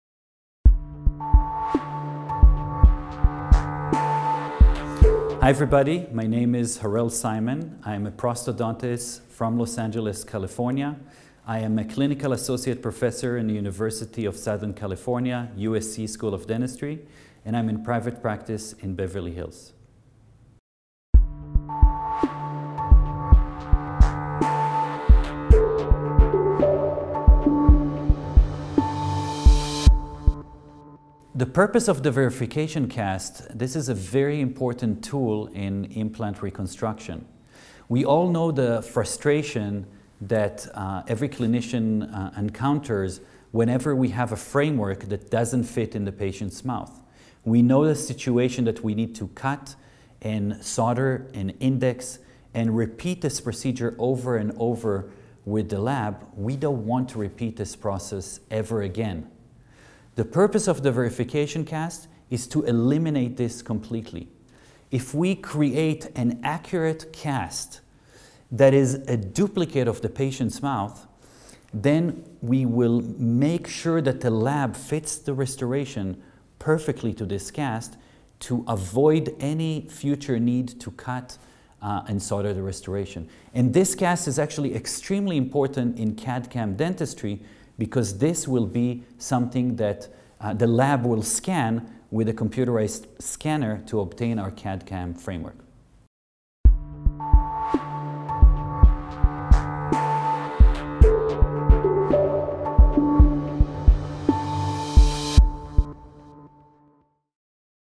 Harel Simon - MANEJO EXITOSO DE LAS COMPLICACIONES EN PRÓTESIS SOBRE IMPLANTES
Harel Simon, prostodoncista de Los Ángeles, California. Profesor Clínico Asociado en la Universidad del Sur de California, en la Facultad de Odontología de la USC. Consulta privada en Beverly Hills, nos responde a la pregunta:

“¿Cuáles son el objetivo del modelo de verificación y los factores clave para obtener un ajuste pasivo de la restauración final sobre los implantes?”